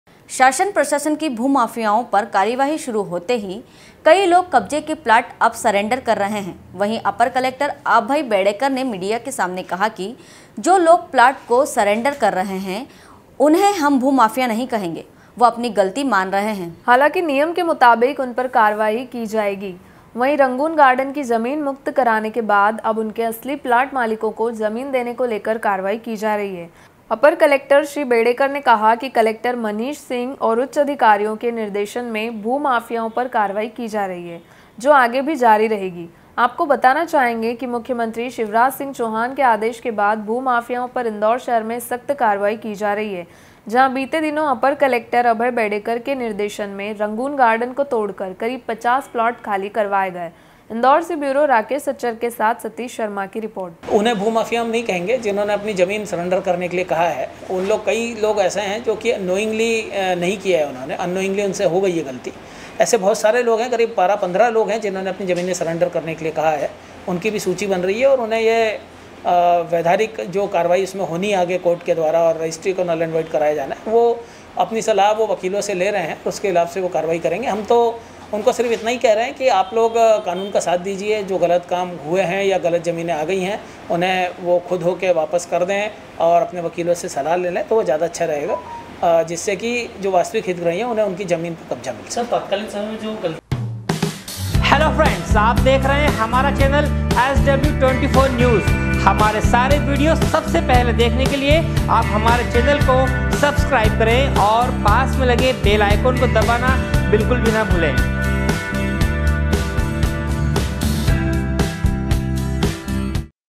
indore- भू माफियाओं पर कार्रवाई शुरू होते ही कई लोग कब्जे के प्लाट अब सरेंडर कर रहे हैं
शासन प्रशासन की भू माफियाओं पर कार्रवाई शुरू होते ही कई लोग कब्जे के प्लाट अब सरेंडर कर रहे हैं वही अपर कलेक्टर अभय बेडेकर ने मीडिया के सामने कहा कि जो लोग प्लाट को सरेंडर कर रहे हैं। इंदौर से ब्यूरो राकेश सच्चर के साथ सतीश शर्मा की रिपोर्ट